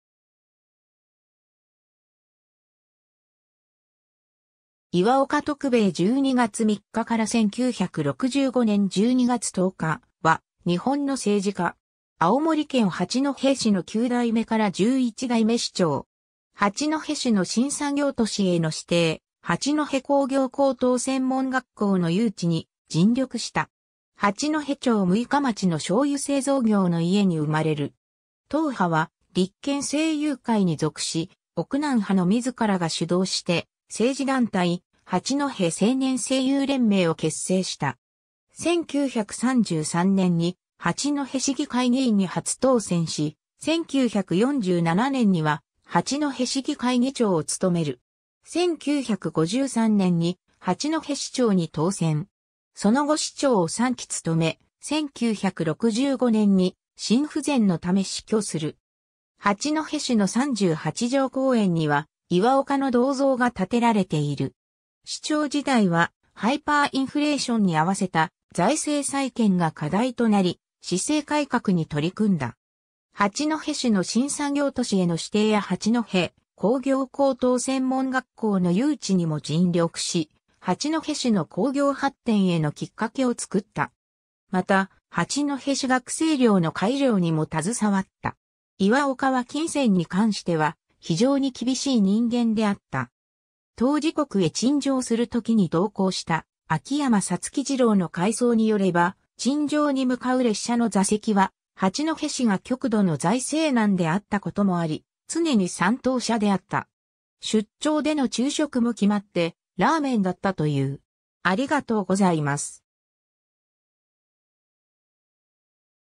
岩岡徳兵衛
岩岡 徳兵衛（いわおか とくべえ、1900年（明治33年）12月3日 - 1965年（昭和40年）12月10日）は、日本の政治家。
青森県八戸市の9代目～11代目市長。
八戸市の新産業都市への指定、八戸工業高等専門学校の誘致に尽力した。
 八戸町六日町の醤油製造業の家に生まれる。
党派は立憲政友会に属し、奥南派の自らが主導して政治団体「八戸青年政友連盟」を結成した。
1933年に八戸市議会議員に初当選し、1947年には八戸市議会議長を務める。
1953年に八戸市長に当選。
その後市長を３期務め、1965年に心不全のため死去する。
 八戸市の三八城公園には岩岡の銅像が建てられている。
 市長時代はハイパーインフレーションに合わせた財政再建が課題となり、市政改革に取り組んだ。
八戸市の新産業都市への指定や八戸工業高等専門学校の誘致にも尽力し、八戸市の工業発展へのきっかけを作った。
また、八戸市学生寮の開寮にも携わった。
 岩岡は金銭に関しては非常に厳しい人間であった。
当時国へ陳情する時に同行した秋山皐二郎（後の八戸市長）の回想によれば、陳情に向かう列車の座席は八戸市が極度の財政難であったこともあり、常に三等車であった。
出張での昼食も決まってラーメンだったという。